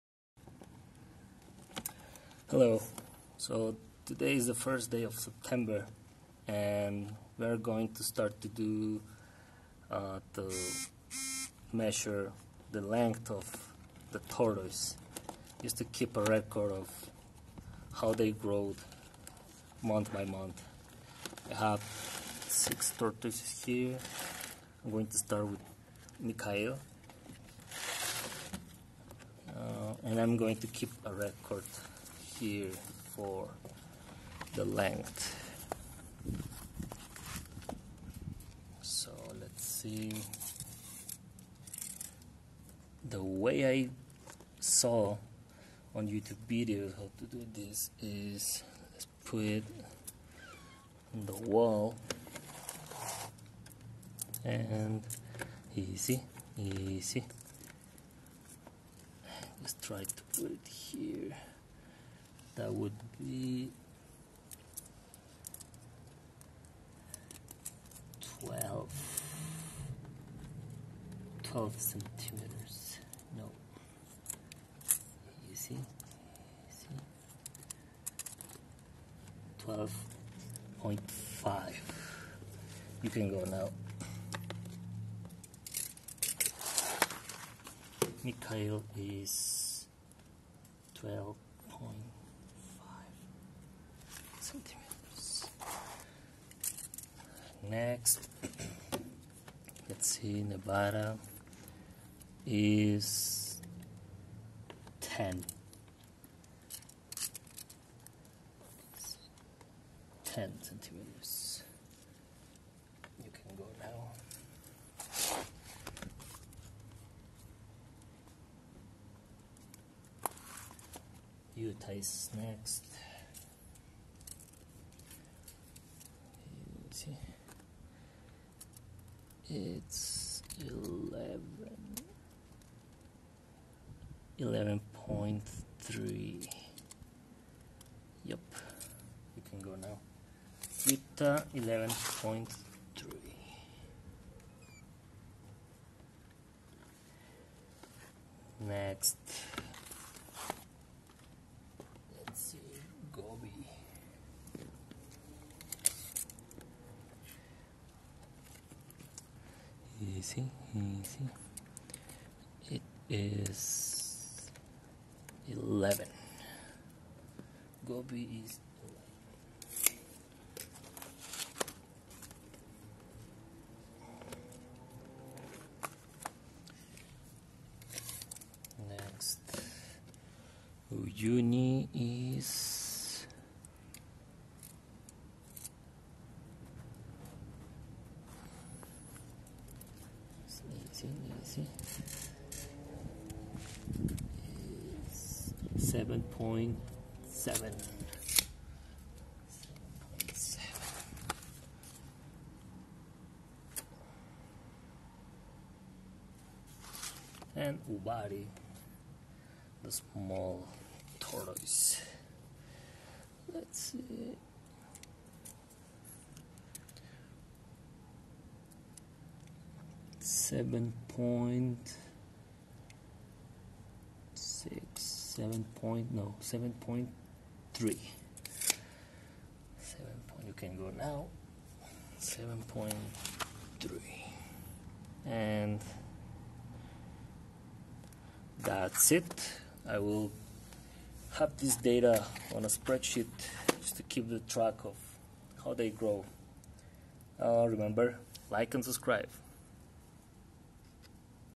Measuring the carapace of 6
Today is the beginning of September and I have decided to measure the carapace of each tortoise to track how they grow month by month. I'll do this the first day of the month and put the data on a spreadsheet.
September 1st 2023 measurements:
Mikhail - 12.5cm
Utah - 11.3cm
Gobi - 11cm
Nevada - 10cm
Uyuni - 7.7cm
Ubari - 7.3cm